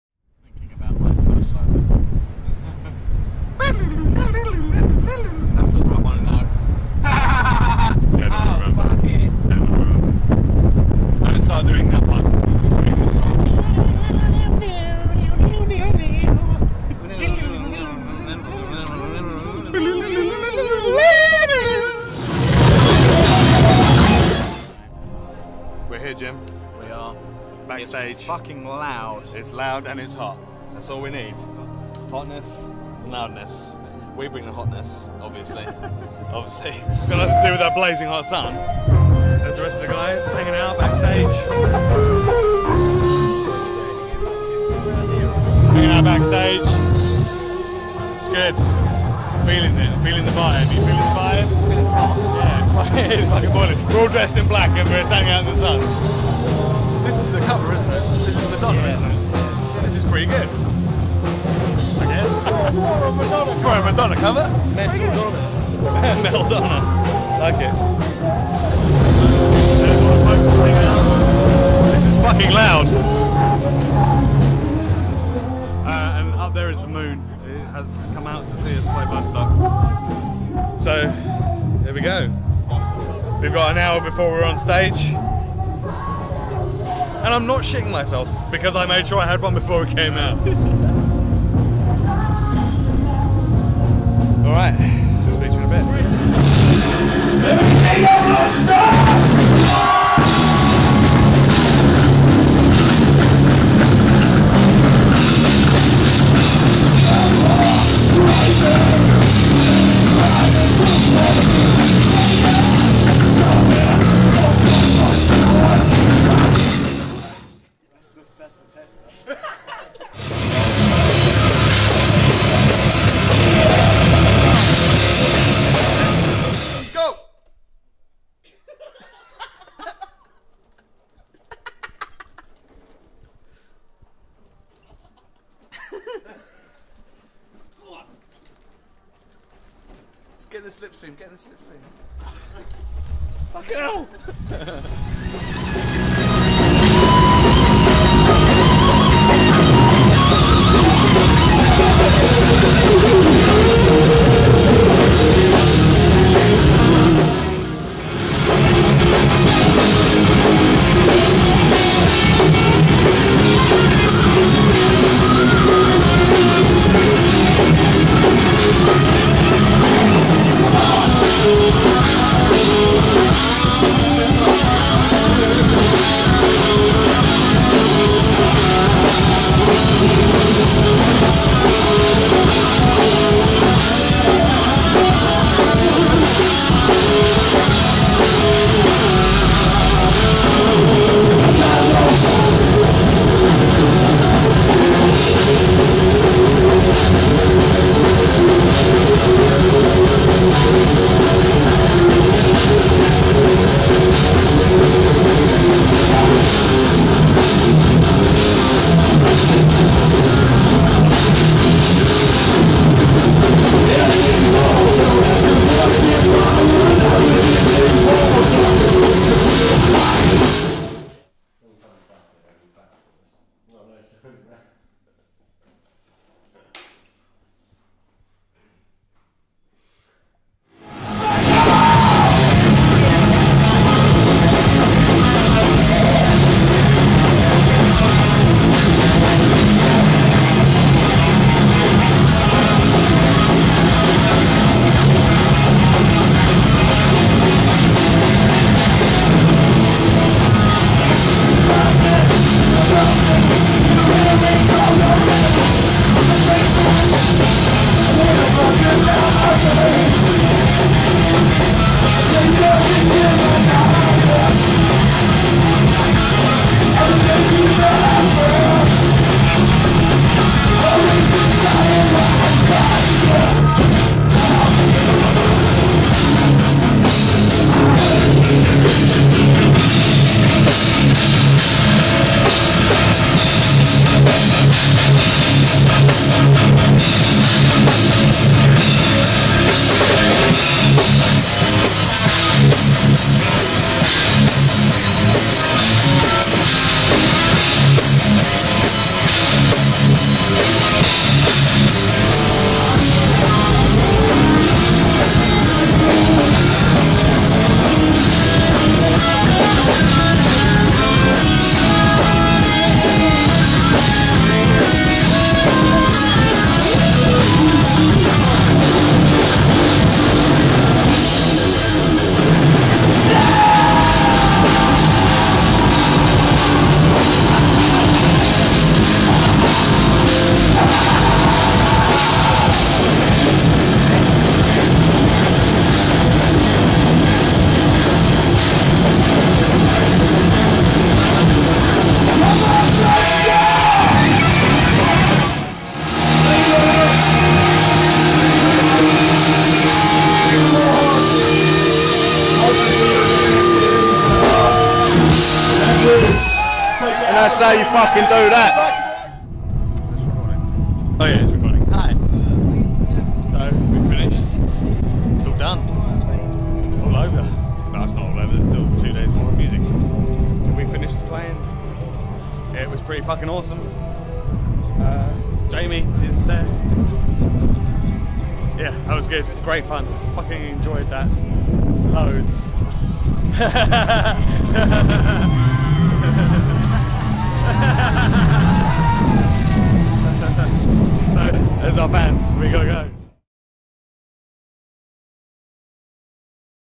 Warhorse Bloodstock 2012 vLog
A glimpse into Warhorse's weekend tearing it up at Bloodstock Open Air 2012.